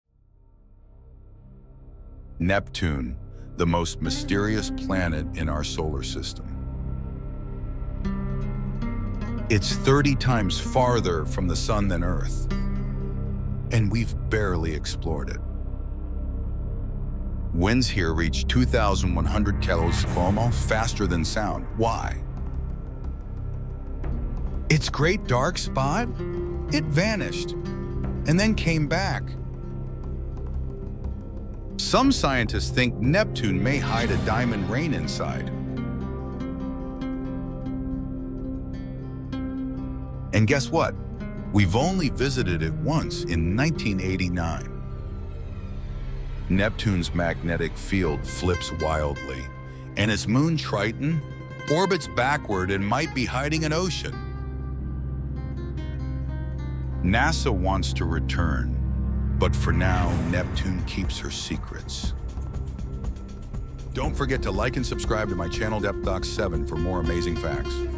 Secrets of Neptune That’ll Blow Your Mind!
🌌 Neptune Secrets REVEALED! 🌀
Dive deep into the mysteries of the solar system’s most distant giant. From wild storms to hidden oceans, discover the mind-blowing facts about Neptune in just 60 seconds! 🔭💙